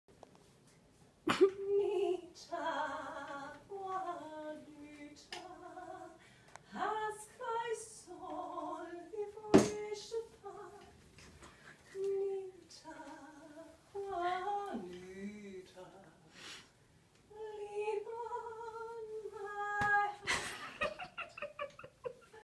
Getting ready for : A Victorian Affair
Just got my dress for a vaudeville/burlesque show I'm singing in this Friday (October 16, at 9PM 1216 N @9th Street Philly PA), and figuring out how I'm going to dance in it.  This is going to be fun:).